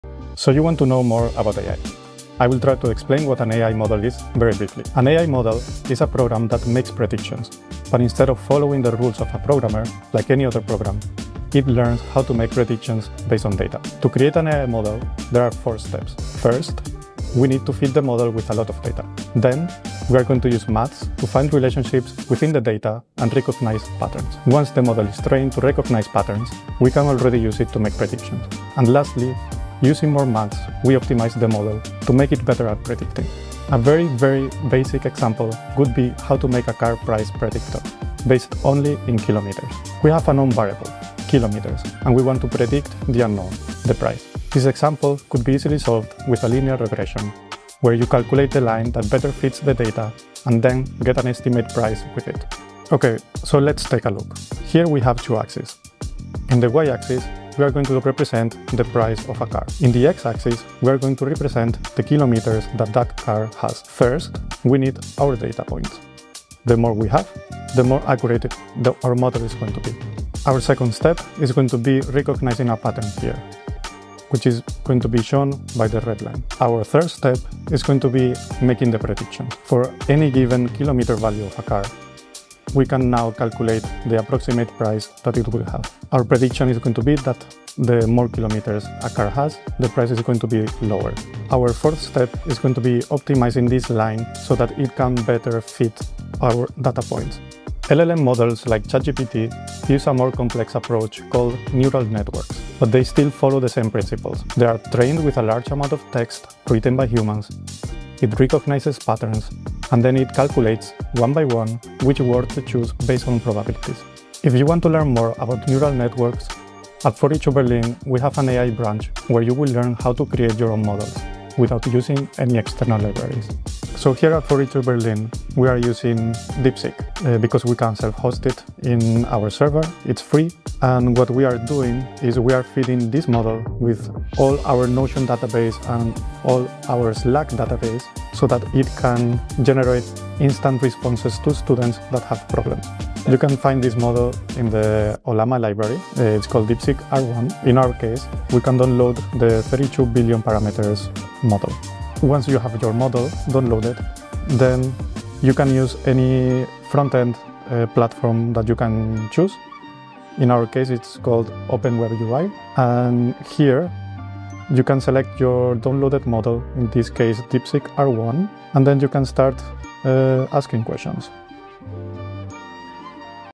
How Do You Build an AI Like ChatGPT?
🧠 Curious about how AI really works?
In this video, learn how to create your own AI model — step by step.

🚀 With Pedro Ruiz, Tech Lead at 42 Berlin, we explore how to build an AI model from scratch:

- Feed it with data
- Use math to identify patterns
- Employ the model for prediction
- Optimize it for better performance

At 42 Berlin, we’ve launched a dedicated AI branch to help students understand and build AI tools themselves.

We’re using Deepsick, available via the Ollama Library under deepseek-r1 — a free, open-source model.
We're training it on our internal Notion and Slack data, enabling it to deliver fast, relevant answers to support our students when they need help.

📌 Chapters:
00:05 – What is an AI model?
00:12 – How to create an AI model
00:34 – Example: building a car price predictor based on mileage
01:38 – Neural networks as LLMs like ChatGPT
01:56 – Deepseek-r1: the 42 Berlin AI model
02:23 – How to use the 42 Berlin AI model

This is real, hands-on AI — not just theory.

👉 Whether you're simply curious or ready to build your own models, this is where you start.